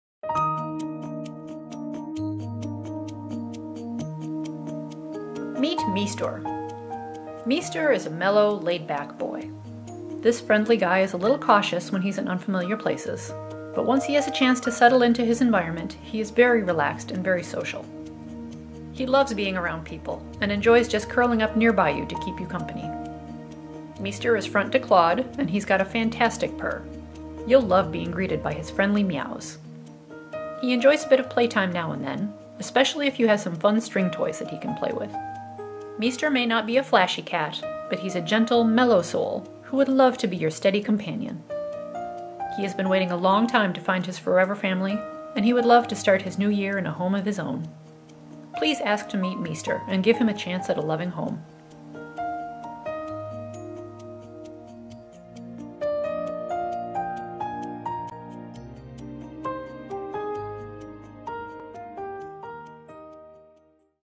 meet Meestor!
Meestor is a mellow, laid-back boy.  He loves being around people, and enjoys curling up nearby you to keep you company.  He has been waiting a long time to find his forever family, and he would love to start his new year in a home of his own.